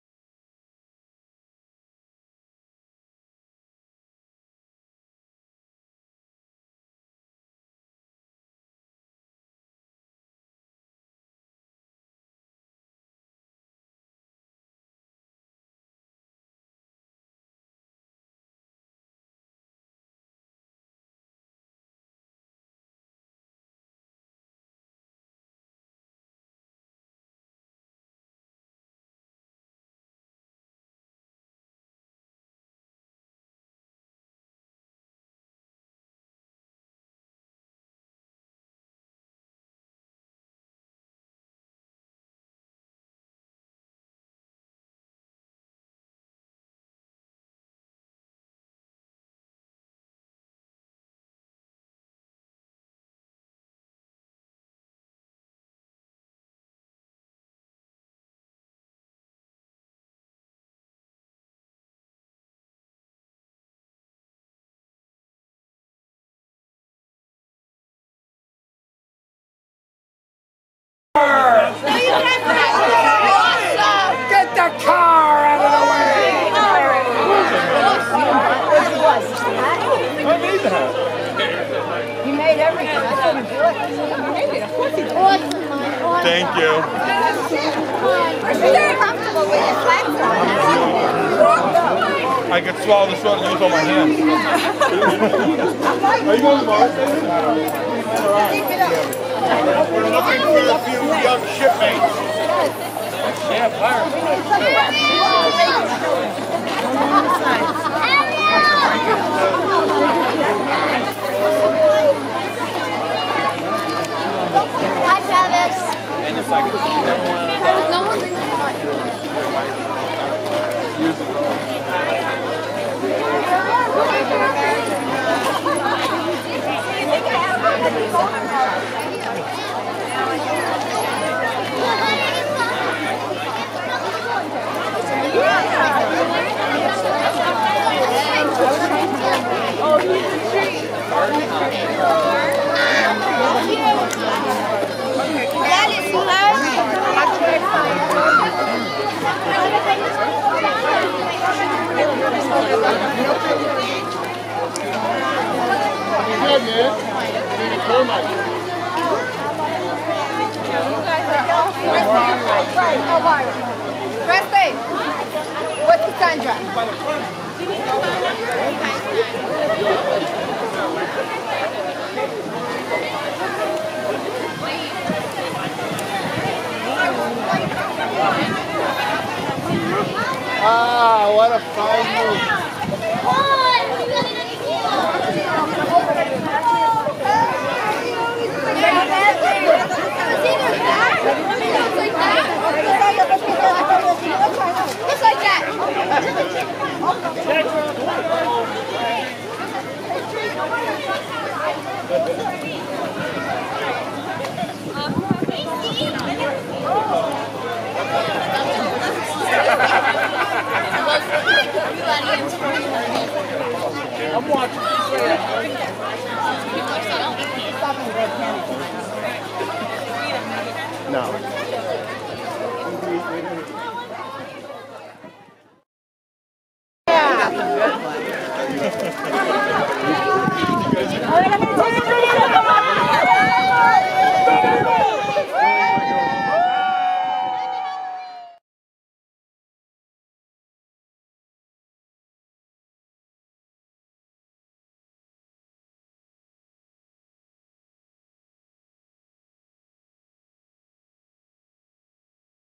City Island Halloween Parade 10 31 2014
Photos and video of the City Island Halloween Parade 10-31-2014.